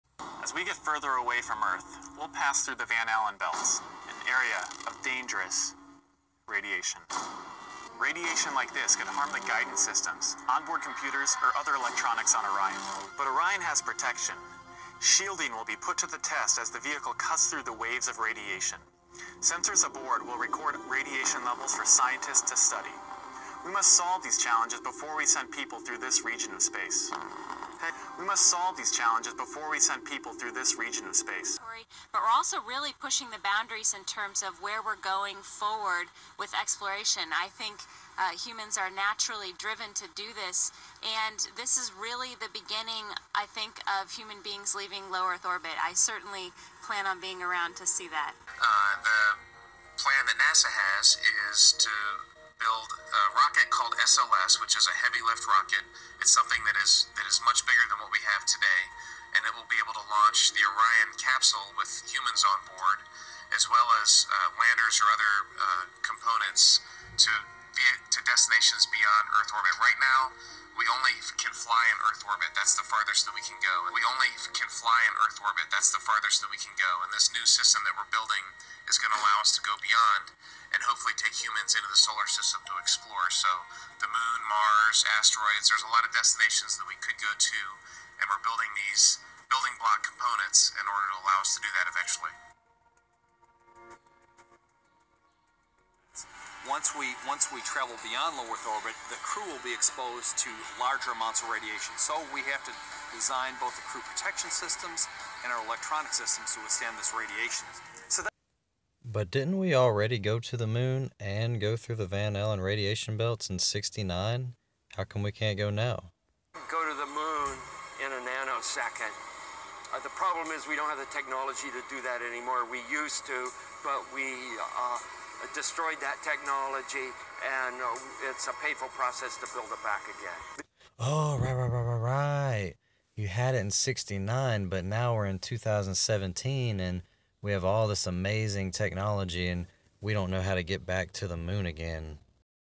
NASA admits we can't go to the moon
Why does NASA say they can't leave the Earth's atmosphere, go through the Van Allen Radiation Belts, or go to the moon when there were a couple supposed landings on the moon in the 70's? 🤔